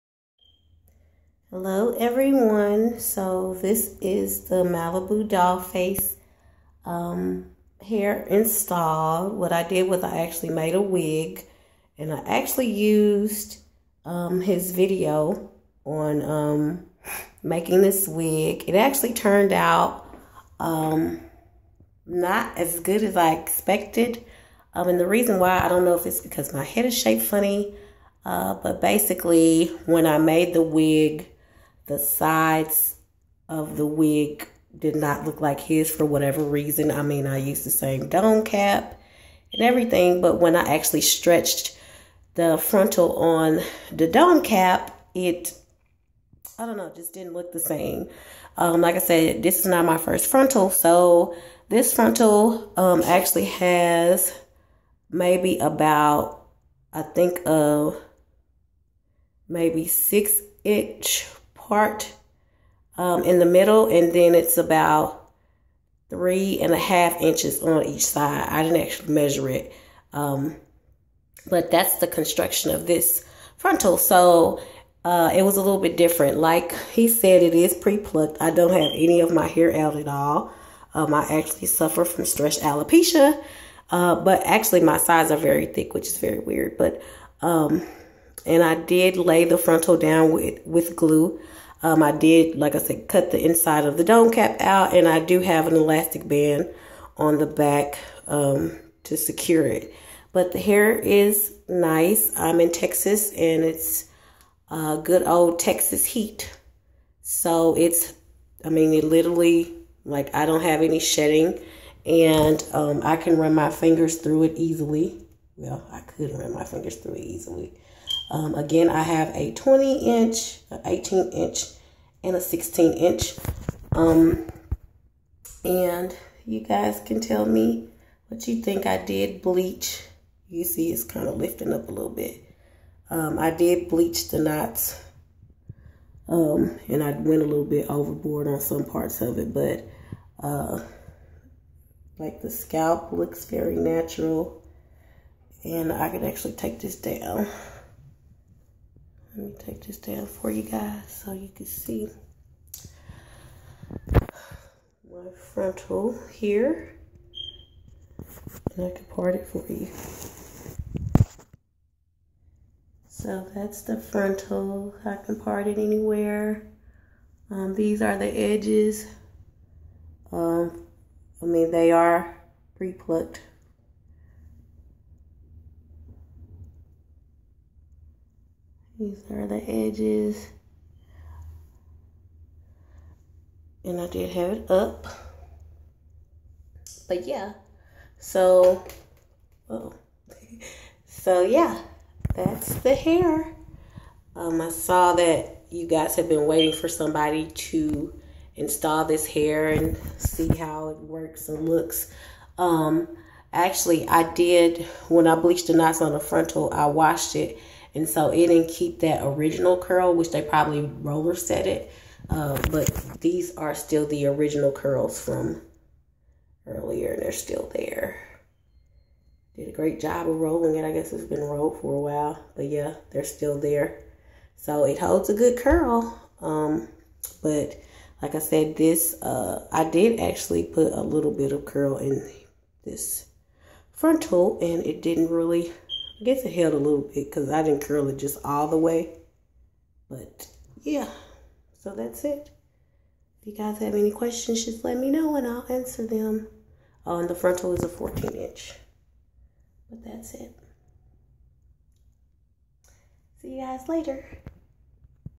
Malibu Dollface Pre-plucked Frontal installed 1 day review
OPEN ME!!!

My first day review of Peruvian Body Way Hot Beauty Hair (Malibu Dollface Preplucked Frontal and bundle wig installation)

This is an honest review for a product that I purchased for full price with my own money. Please feel free to mention when you purchase that you watched my video and receive a discount towards your purchase.

Please let me know if you have any questions, comments, or concerns.